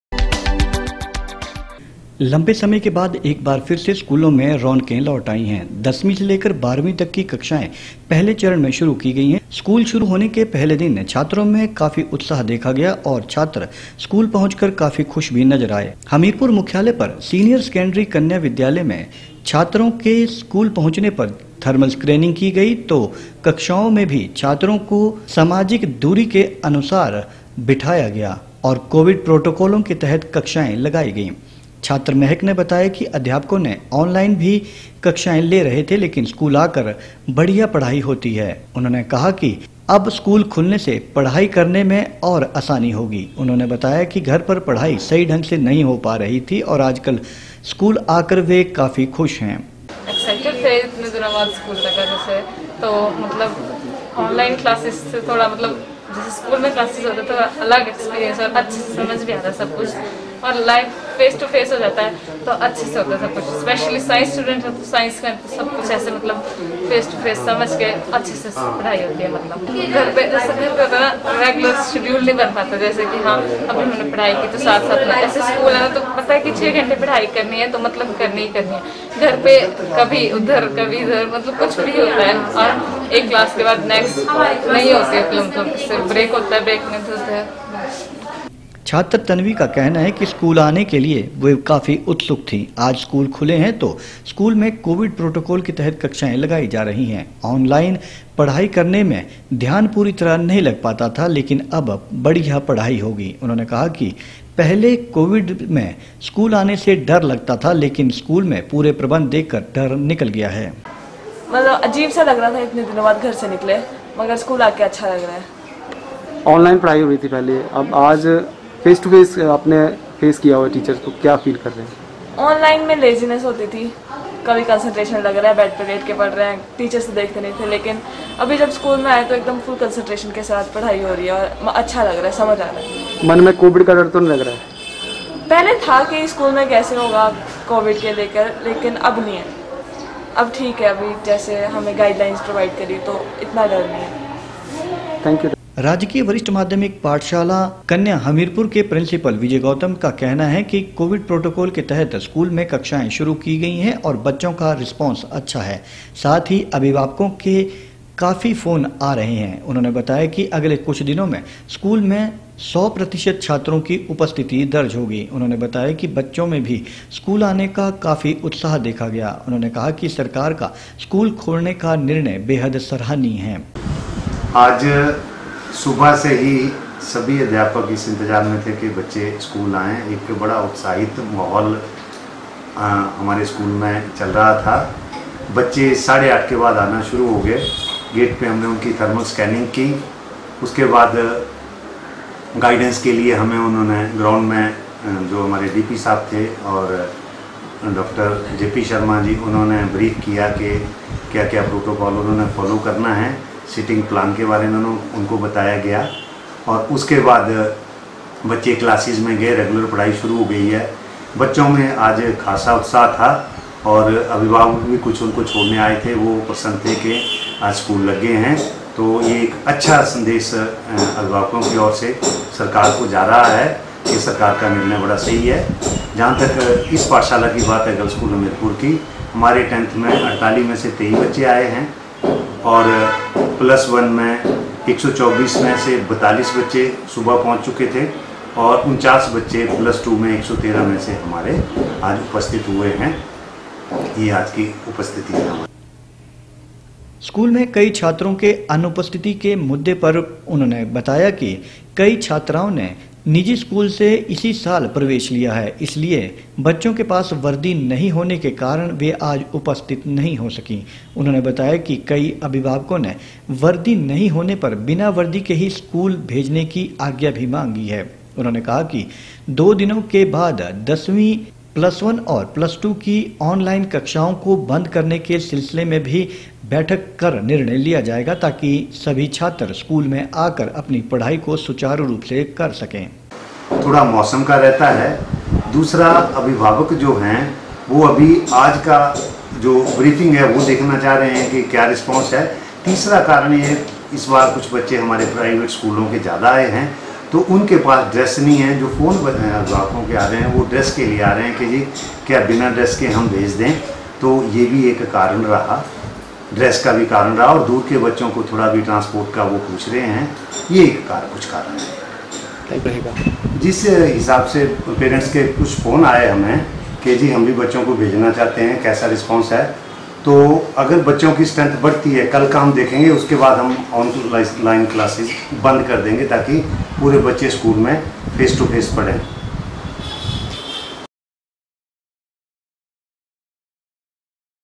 Hamirpur School Opening कन्या स्कूल में छात्राओ के पहुंचने पर की गई थर्मल स्क्रीनिंग
लंबे समय के बाद एक बार फिर से स्कूलों में रौनक लौटी
स्कूल शुरू होने के पहले दिन छात्रों में काफी उत्साह देखा गया और छात्र स्कूल पहुंचकर काफी खुश नजर आए
कन्या स्कूल में छात्राओ के पहुंचने पर की गई थर्मल स्क्रीनिंग
कक्षाओं में भी कोविड प्रोटोकाल का रखा गया पूरा ध्यान।
प्रिसीपल विजय गौतम ने कहा दसवीं, ग्यारहवीं और बाहरवीं की आनलाइन कक्षाएं की जाएगी बंद।